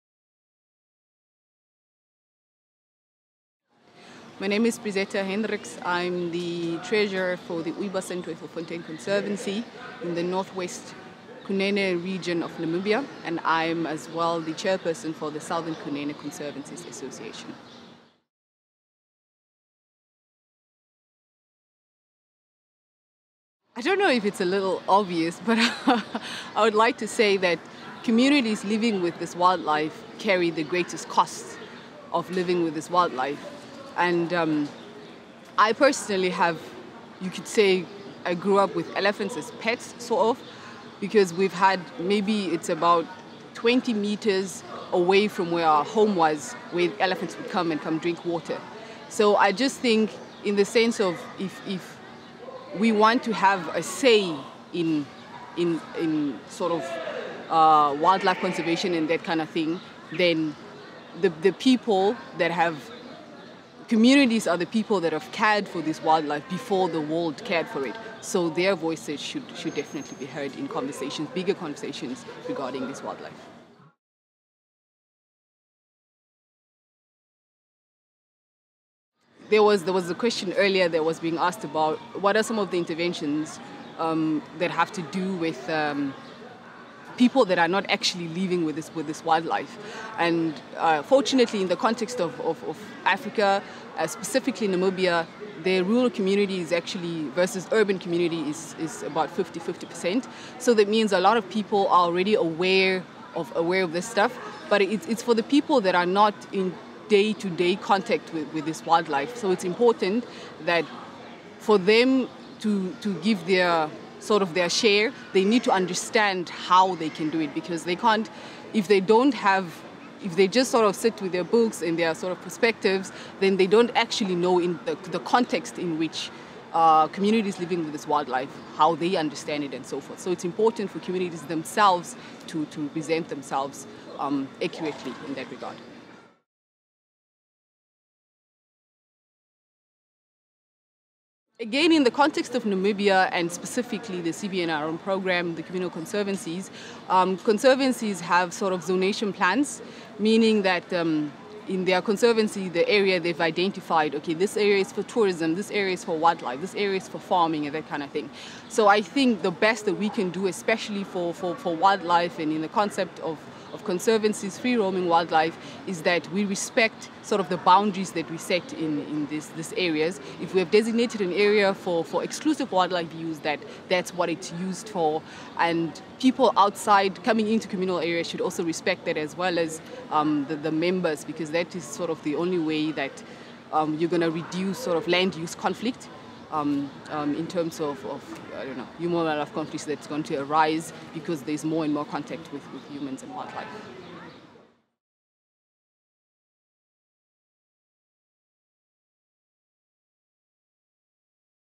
Community Voices on IWT: Brisetha Hendricks
Brisetha Hendricks, Ûibasen Twyfelfontein Conservancy, Namibia, is interviewed about the importance of local communities getting a voice in efforts to combat the illegal wildlife trade.

Hendricks was among the participants at a Community Voices: local perspectives on international responses to the illegal wildlife trade conference at London Zoo on 10 October 2018.

The event was organised by IIED, International Union for Conservation of Nature Sustainable Use and Livelihoods Specialist Group (IUCN SULi), Fauna & Flora International (FFI) and the Zoological Society of London (ZSL).

The representatives from communities affected by the illegal wildlife trade called on governments meeting at the London conference on the illegal wildlife trade (11-12 October) to recognise local communities as equal partners in conserving wildlife, strengthen their rights to land and increase benefits from wildlife, and give local knowledge equal status in tackling the illegal wildlife trade.